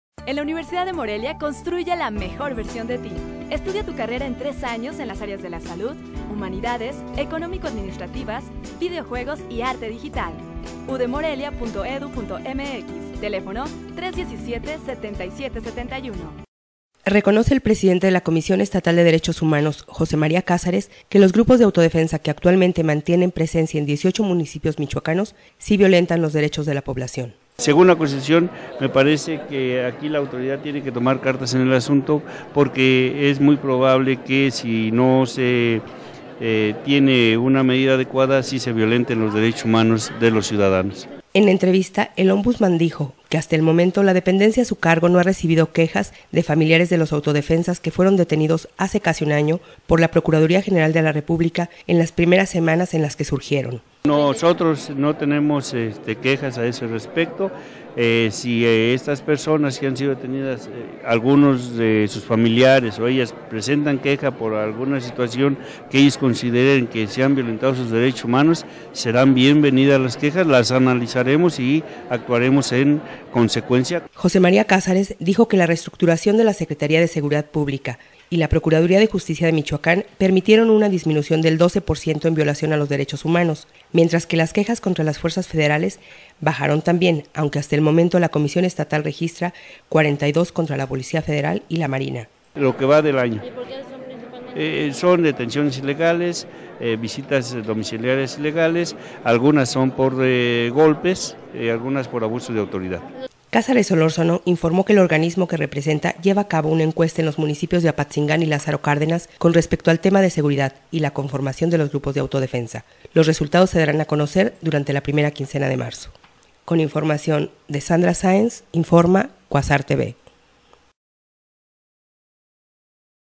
Autodefensas violentan derechos humanos de la población
Reconoce el presidente de la Comisión de Derechos Humanos, José María Cázares que los grupos de autodefensa que actualmente mantienen presencia en 18 municipios michoacanos sí violentan los derechos de la población...